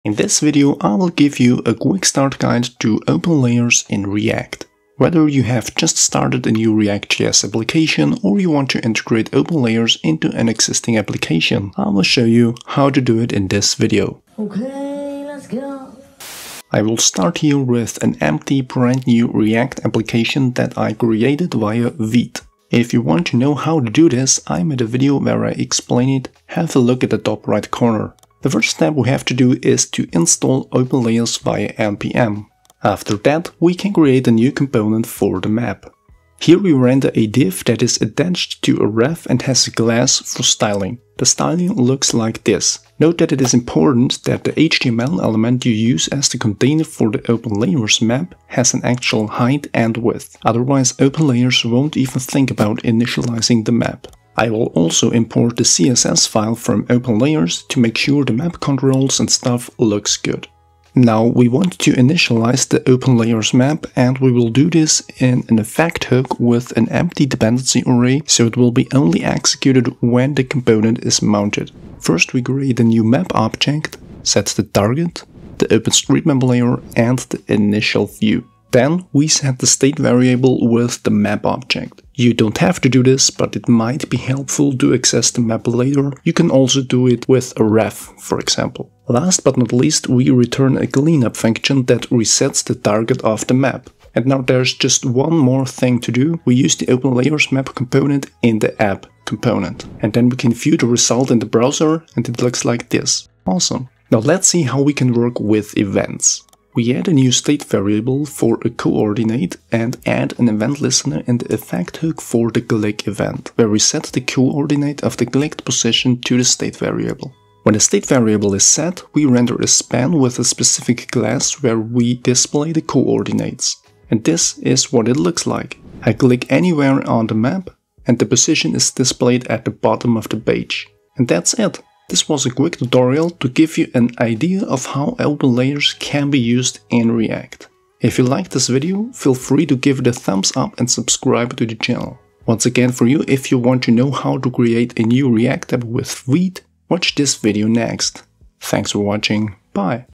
How to get started with OpenLayers in React JS
Watch this video, if you want to know how to integrate OpenLayers in a React web application. I promise, it's very easy and super simple to follow ;)